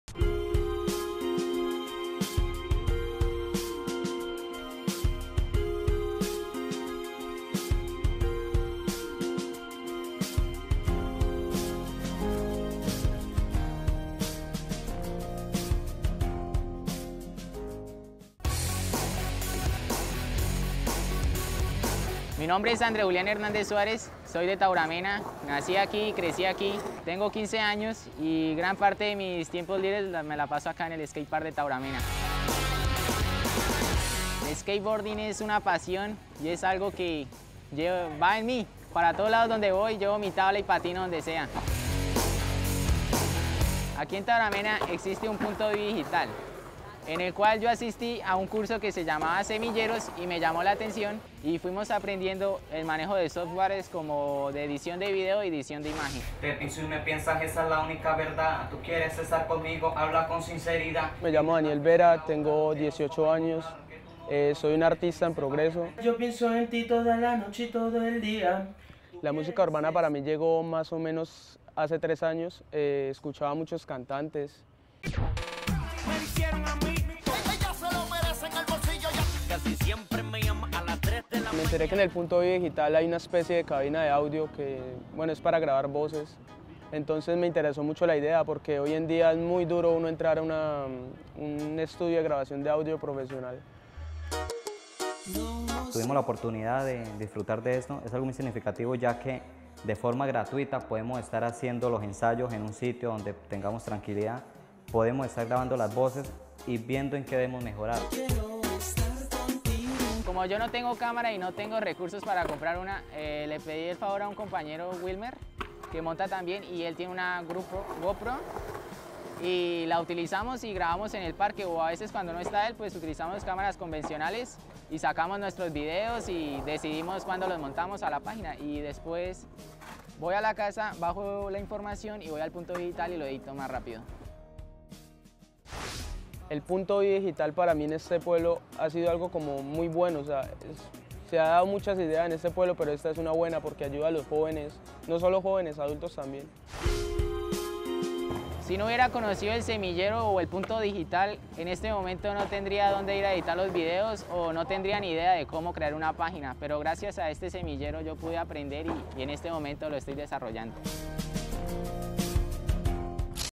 Semilleros audiovisuales en Tauramena C18-N2
En Tauramena, Casanare, los jóvenes se capacitan en producción audiovisual en el Punto Vive Digital para complementar sus aficiones, documentar sus habilidades deportivas y artísticas.